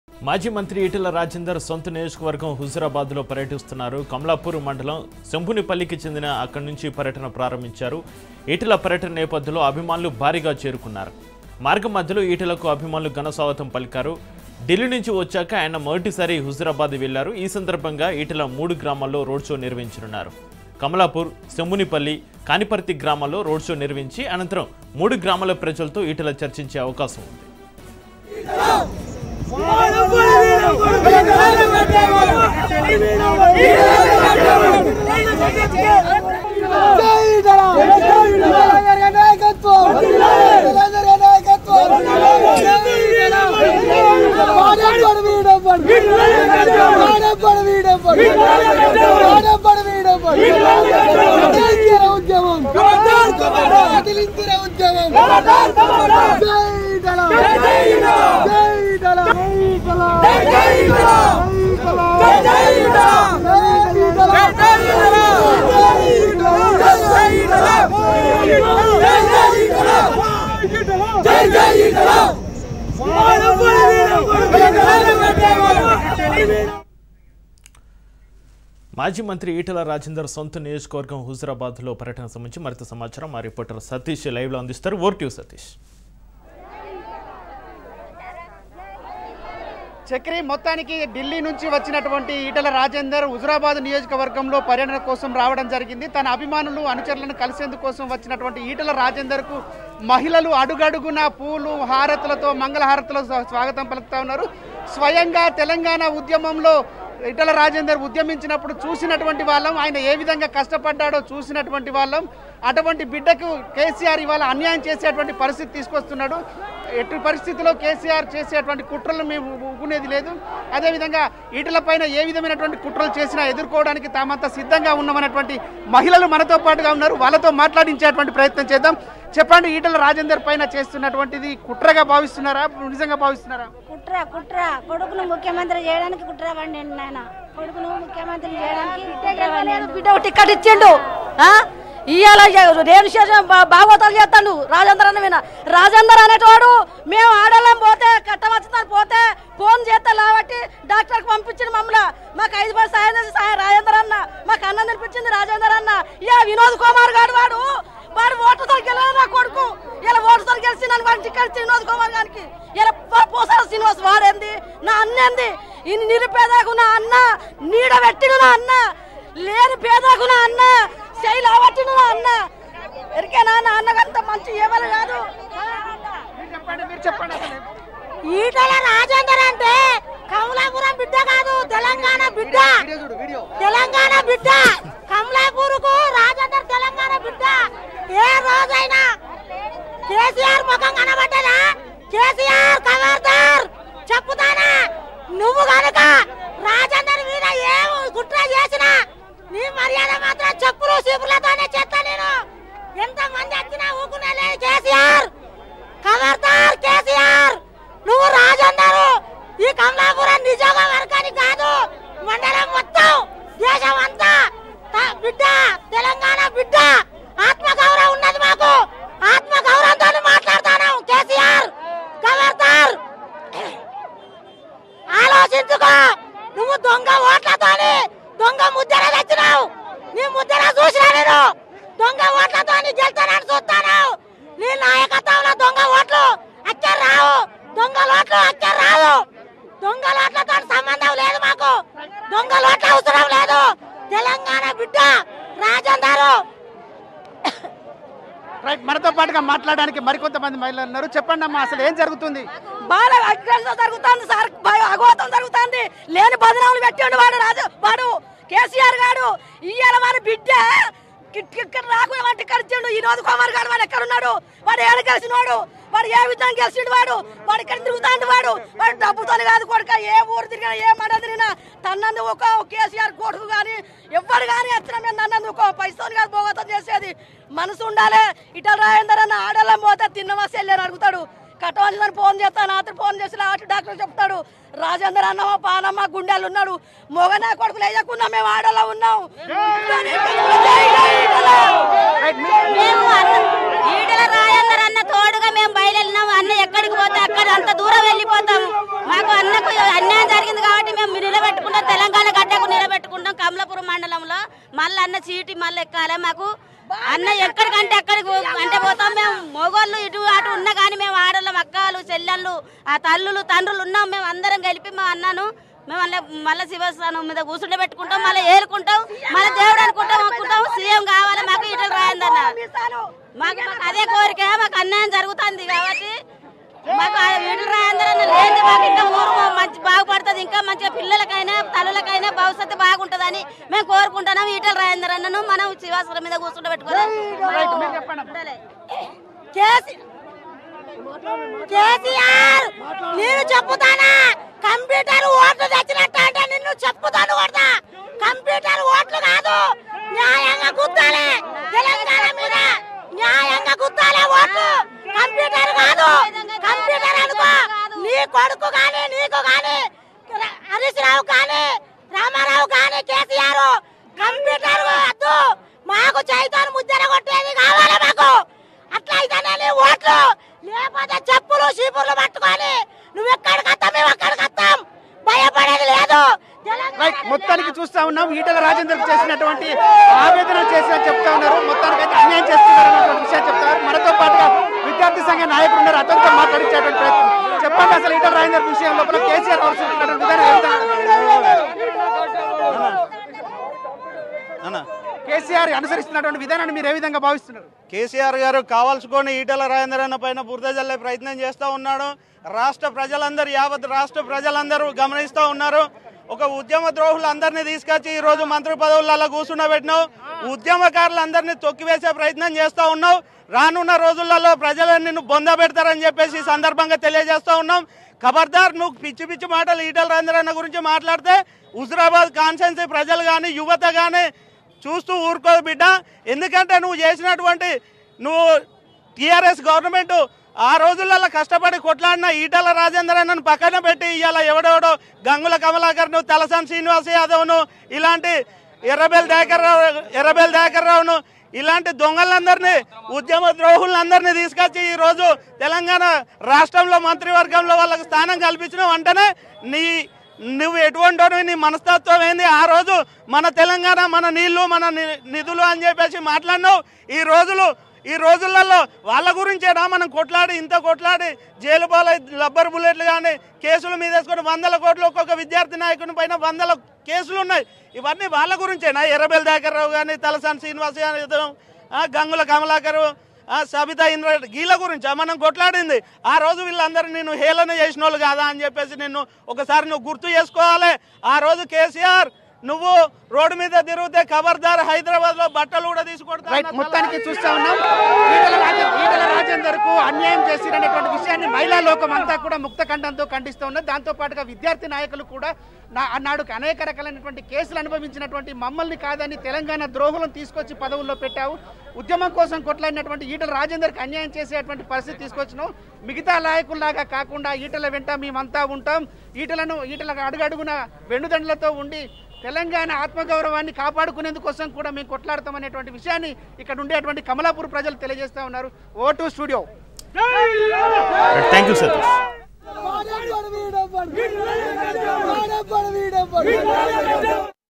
ఈటల పర్యటనకి అడుగడుగునా ఘనస్వాగతం | Etela Rajendar At RoadShow Huzurabad Constituency |RajNewsTelugu
ఈటల పర్యటనకి అడుగడుగునా ఘనస్వాగతం | Etela Rajendar RoadShow Huzurabad Constituency | Raj News Telugu